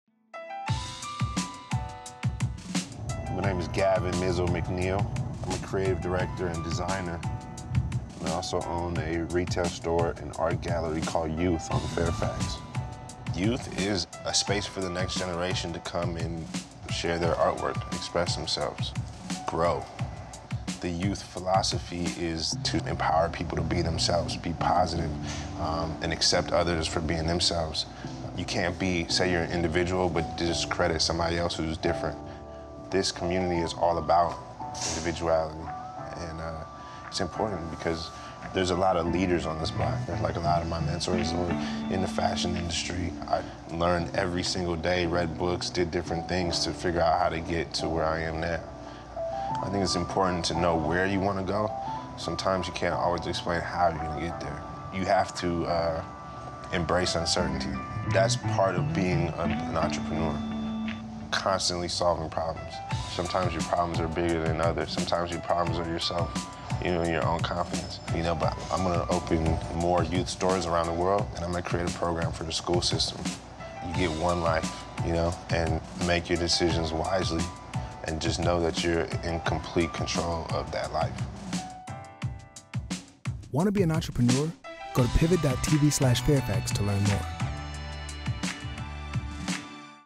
This Fashion Designer Cruises Down Fairfax Encouraging Entrepreneurship
Inspired by the hard work and hustle of the entrepreneurs featured in the Pivot TV show 'Welcome to Fairfax' (Fridays at 10 p.m.), Cadillac and Pivot TV collaborated with Gavin "Mizzle" McNeill, fashion designer and creative visionary behind Just Be Cool, to hit the streets of Los Angeles to meet entrepreneurs making waves and embodying the spirit of the Fairfax community. This video series takes a deep dive into the careers of five young people, explores what it took for them to get where they are today, and showcases how they are working toward what's next.

Watch the video now, and check out more from our Entrepreneurs series